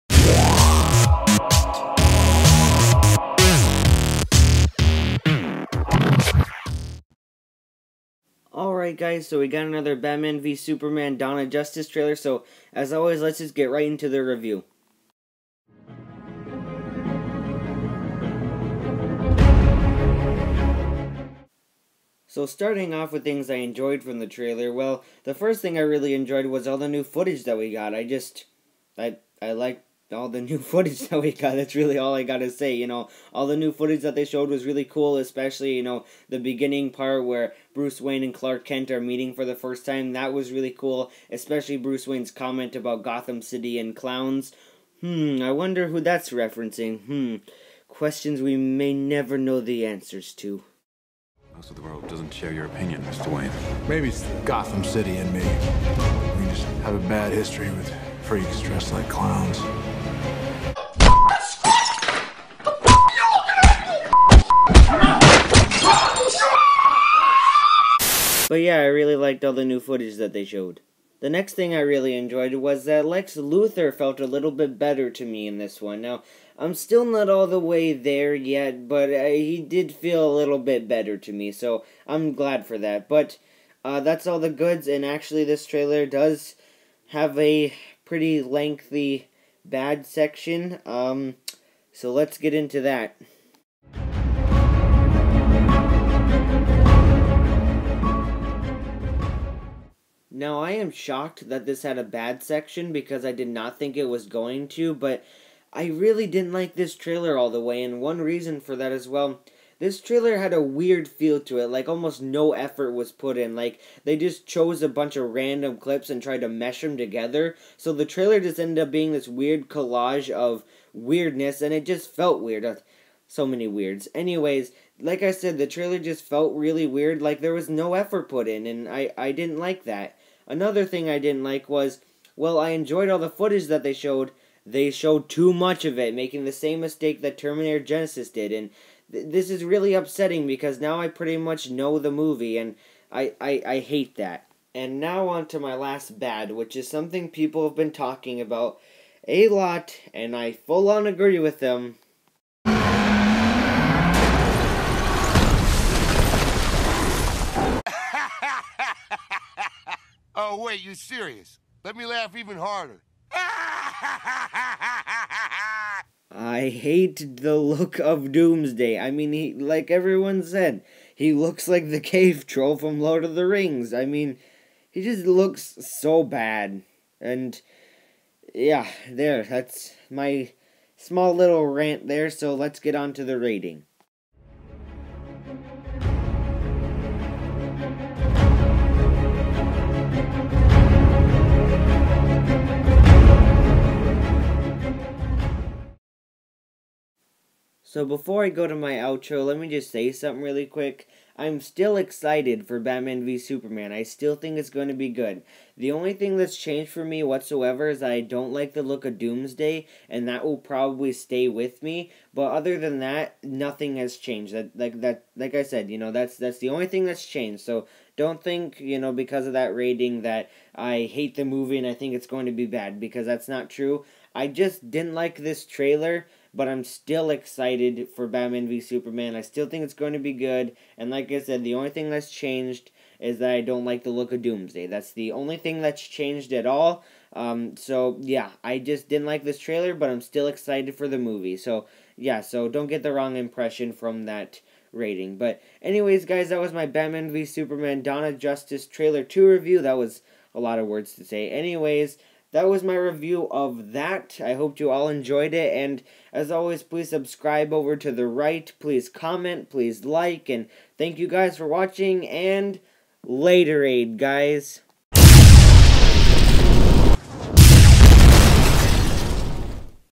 Rock'n Review: Batman V Superman: Dawn Of Justice - Trailer 2
KR rocks gives his review of the new batman v superman trailer.